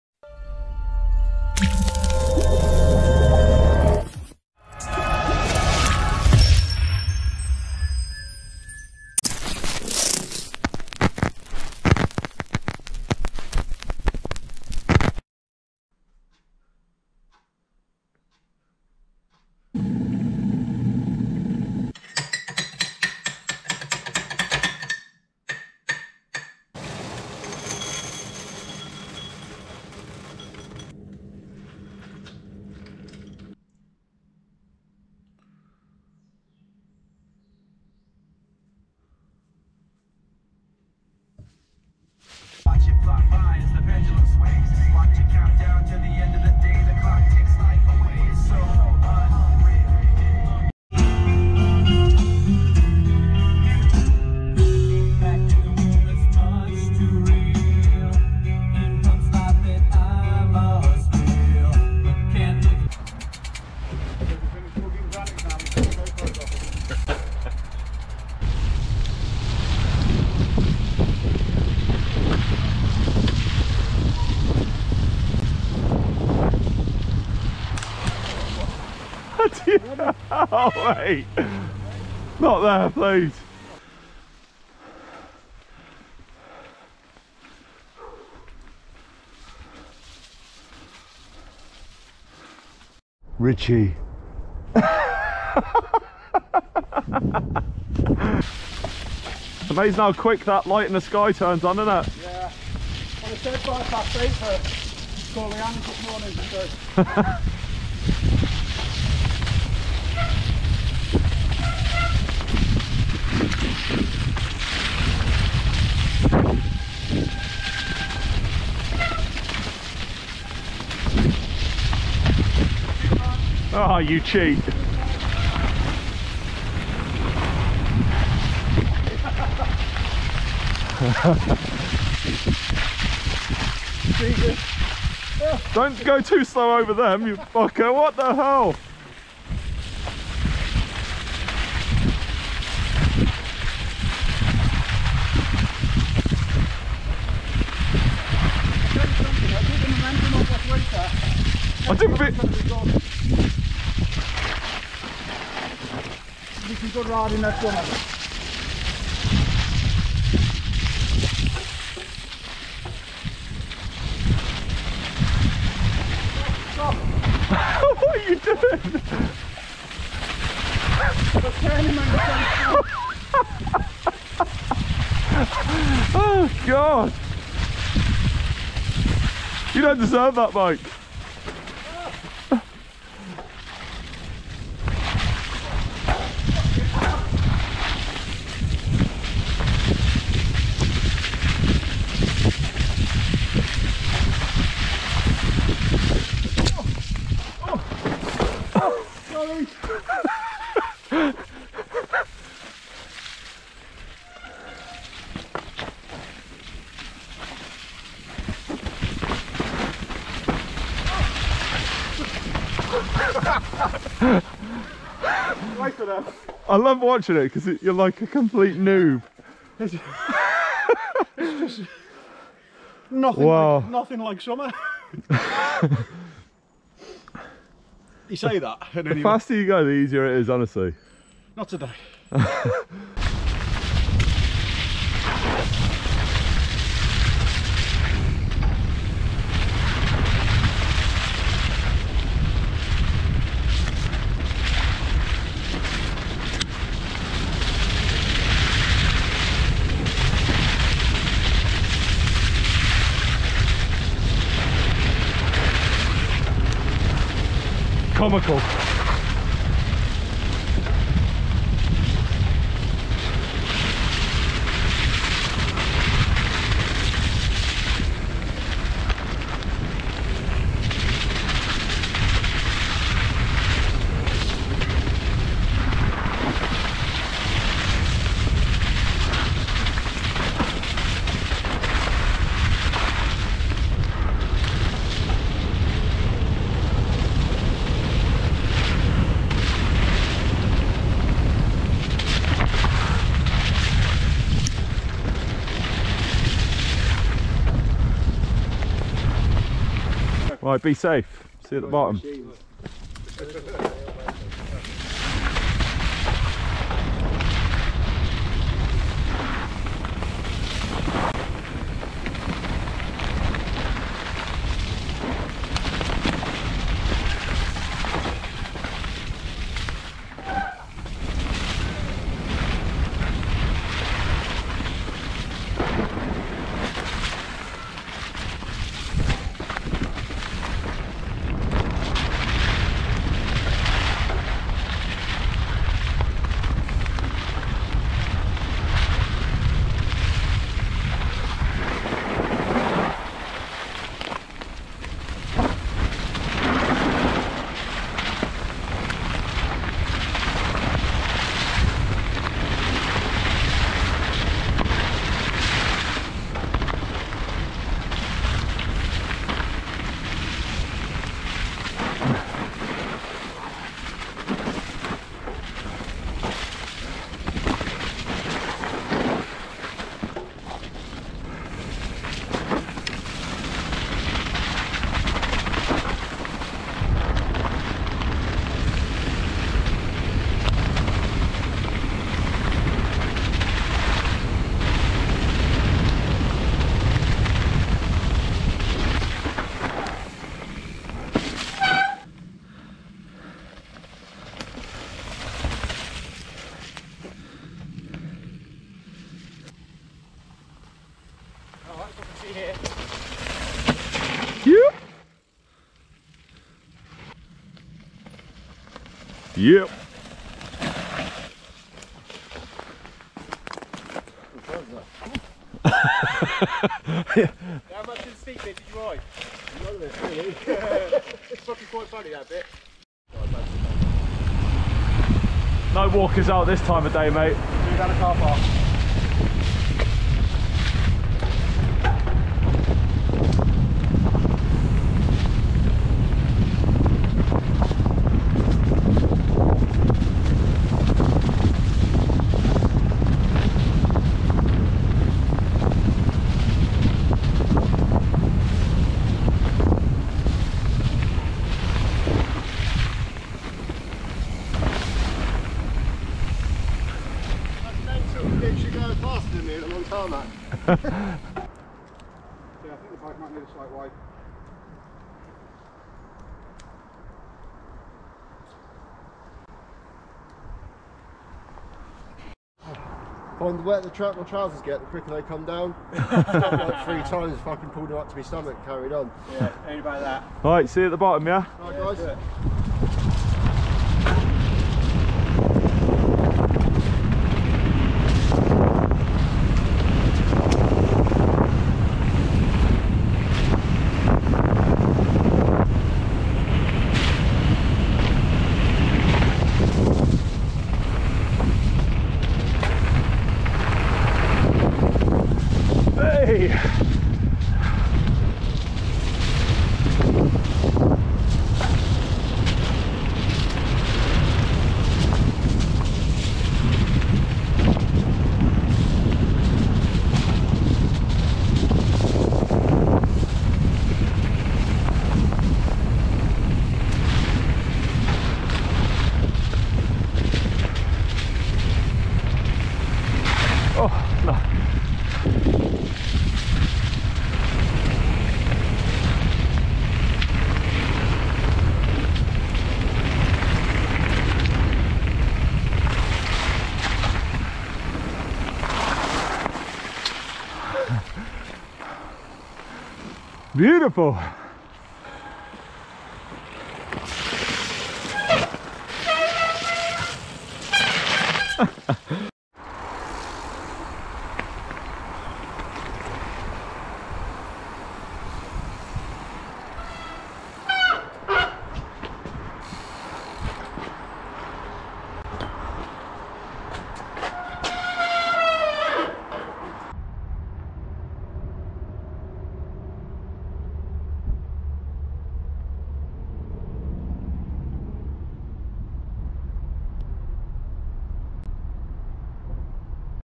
QECP Winter Riding - Testing the GoPro 8
QECP is my local and I spend many hours up there a week riding. After even a slight bit of rain, it's amazing how much the dynamic of the area changes. Roots, leaves and general ground conditions all adding to a varied ride. That said, it's exactly why we ride it. Getting soaked through and covered in mud is all part of the adventure. It certainly makes you a better rider throughout the winter months. Come summer time we should all be on fire.

What makes these trips fantastic are the people you ride with. We have such a laugh and the banter is non stop. It's the way we motivate each other during these colder/wetter months.

There's such a variety of trails at QECP and even though I've been riding there for about 4 years now, it's still a challenge everytime.

Oh, I was also testing out a GoPro 8. Not impressed with the wind settings. Didn't seem to do anything really. So I need to think how to get round that one.

My FaceBook Group (MTB Fanatics)

Flowvision UK

Flowvision USA